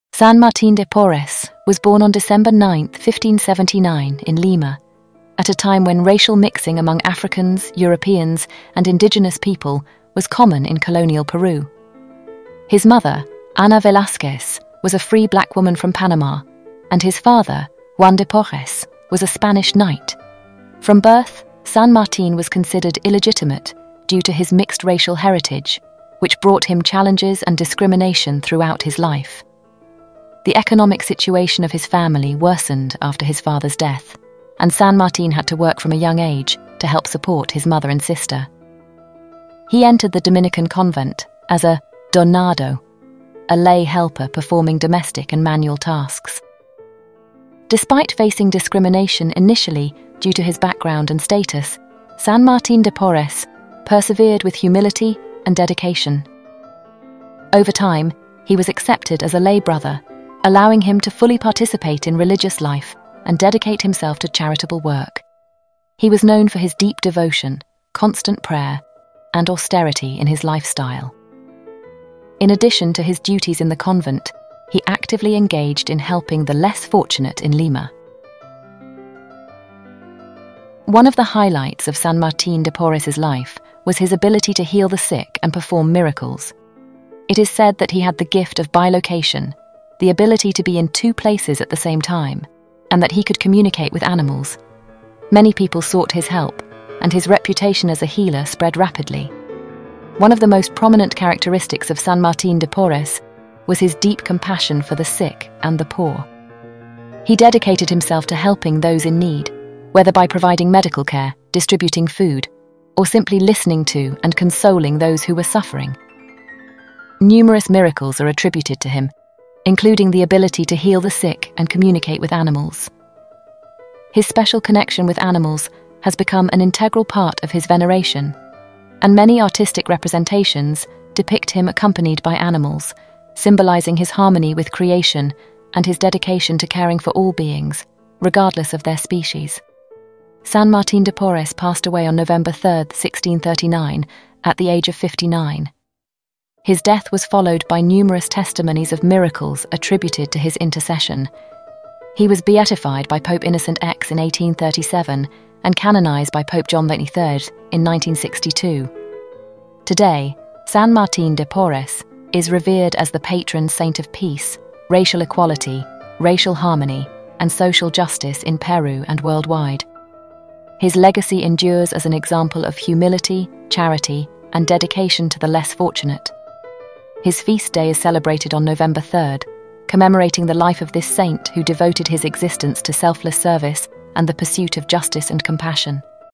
St Martin de Porres 🙏🏻🤍 ✝️🐶
San Martín de Porres was born on December 9, 1579, in Lima, Peru, during the time of Spanish colonial rule. He was the illegitimate son of a Spanish nobleman, Juan de Porres, and a freed slave woman from Panama, Ana Velázquez. San Martín grew up in poverty and faced discrimination due to his mixed-race heritage.

From a young age, San Martín showed a deep compassion for the poor and the sick. Despite facing challenges and prejudice, he was determined to dedicate his life to serving others. At the age of 15, he joined the Dominican Order as a lay brother, working as a servant and performing menial tasks in the monastery.

San Martín's humility, piety, and selflessness soon became evident to his fellow friars and to the people of Lima. He devoted himself to prayer, fasting, and serving the poor, often going beyond the call of duty to help those in need. He cared for the sick, fed the hungry, and provided comfort to the suffering.

San Martín's reputation for holiness and miraculous deeds grew over time. It was said that he had the gift of healing, and many people sought his intercession for their ailments. He was also known for his ability to communicate with animals and for his deep love and compassion for all living creatures.

Despite his lowly position in society, San Martín became a beloved figure in Lima and beyond. He was known as a miracle worker and a symbol of hope for the poor and the marginalized. Countless stories of his miracles and acts of charity spread throughout Peru and beyond.

San Martín de Porres died on November 3, 1639, at the age of 59. He was canonized as a saint by Pope John XXIII in 1962, becoming the first black saint from the Americas. Today, he is venerated as the patron saint of racial harmony, social justice, and those who work for the poor and the oppressed. His feast day is celebrated on November 3rd each year, commemorating his life and legacy of love, compassion, and service to others.